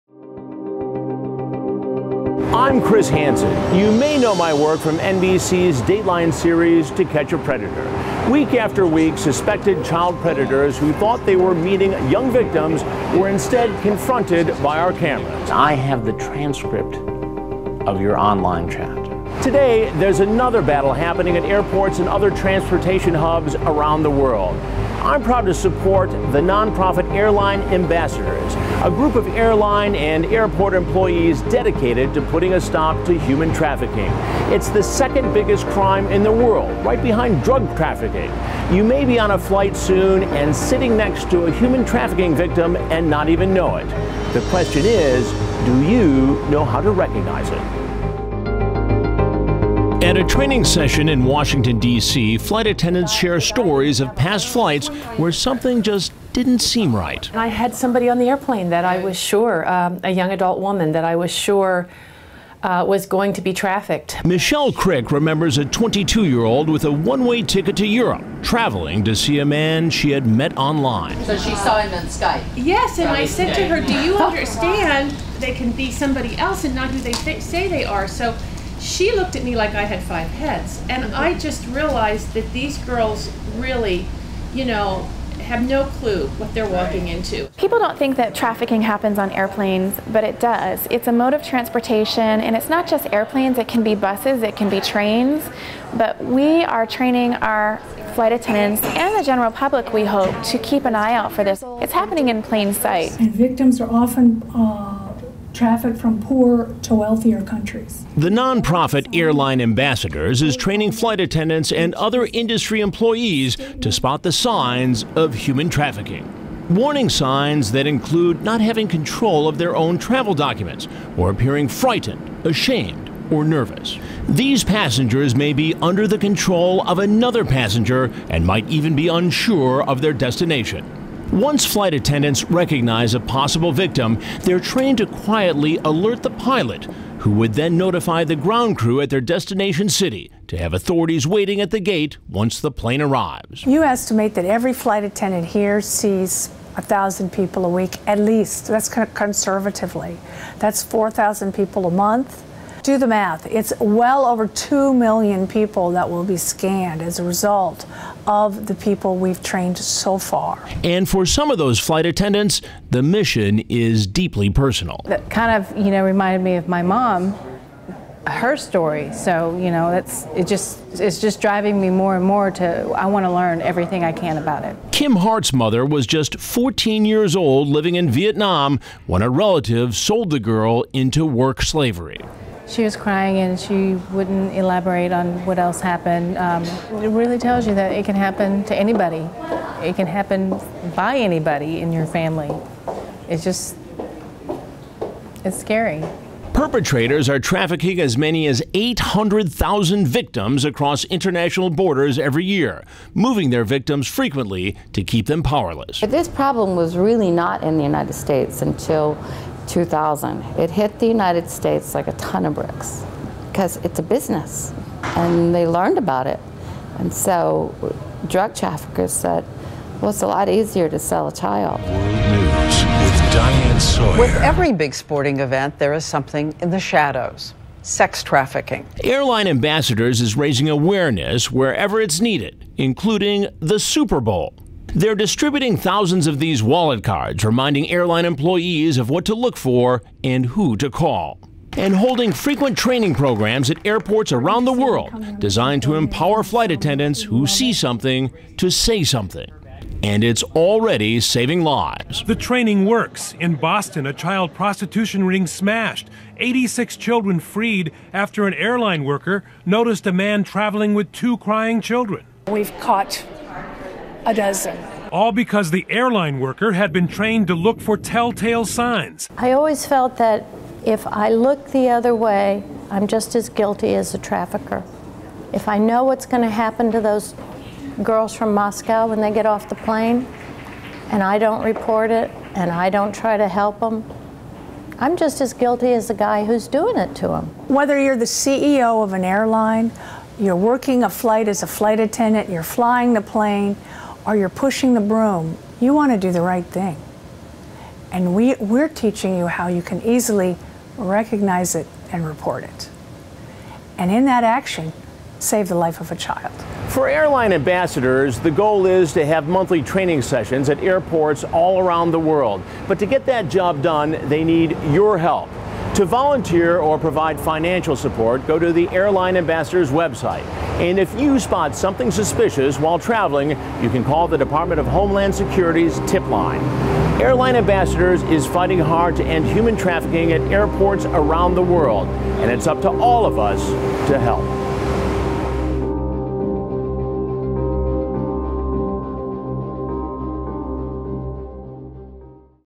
"To Catch a Predator" host Chris Hansen for Airline Ambassadors: How to Spot Human Trafficking
Former "Dateline NBC: To Catch a Predator" host Chris Hansen is proud to support the human trafficking awareness efforts of Airline Ambassadors.

Airline Ambassadors is actively working to train flight attendants and other airline and airport employees to recognize the signs of human trafficking on airplanes and at airports, and how to report suspected cases.

As Chris explains in this report, it's an effort that has already saved lives.